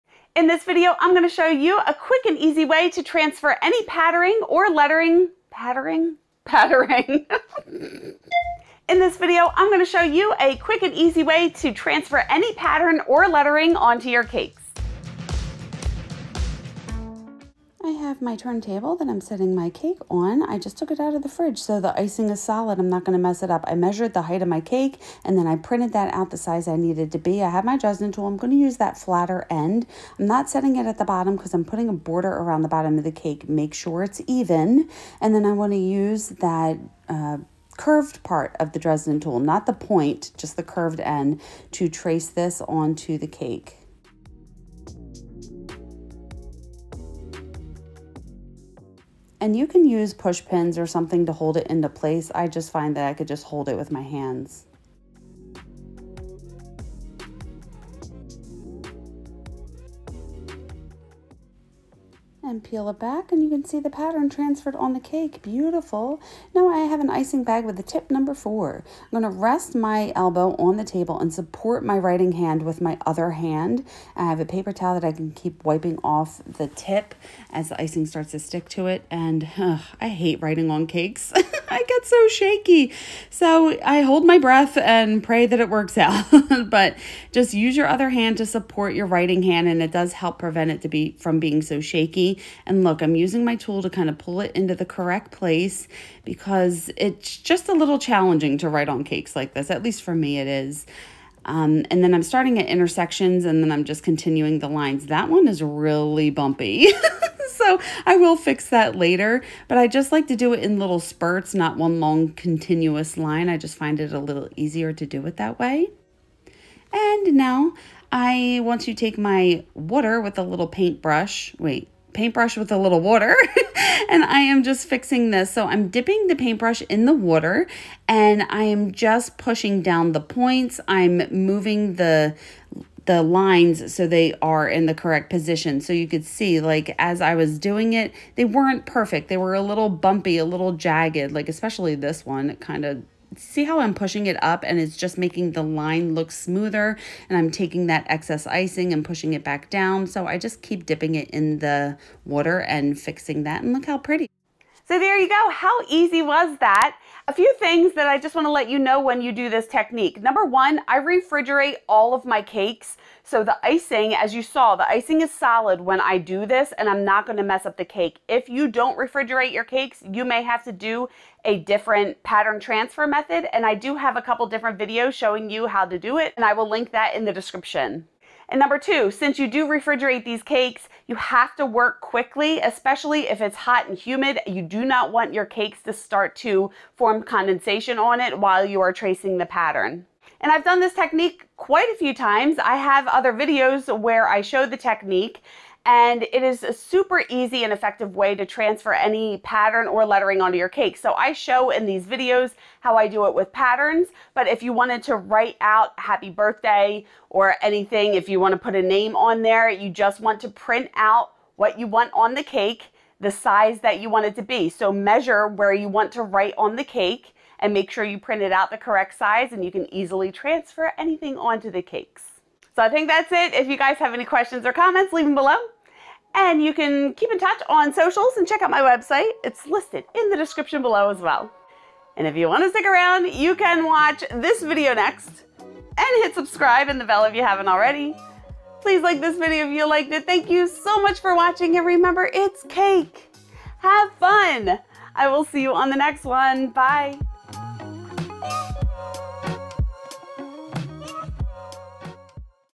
Easily Transfer Any Pattern To Your Cakes!
I figured out a super simple way to transfer any pattern onto your cakes and in this video, I am showing you my technique.  This pattern transfer method will work on any refrigerated buttercream or fondant cakes!

TOOLS I USED (some items are not exactly what I used, but a good substitute):

NON SKID PAD ON TURNTABLE

SMALL RULER

DRESDEN TOOL (there's one in this set)

ICING BAG

TIP NO 4

(my 2nd IG account since my first one is giving me problems.)

If you have read all of this, thank you!  I really appreciate your support!
I read all comments and try to respond the best I can. 😊
Xoxo,
Karolyn

Karolyn’s Kakes provides cake decorating tutorials and tips to help you make amazing cakes.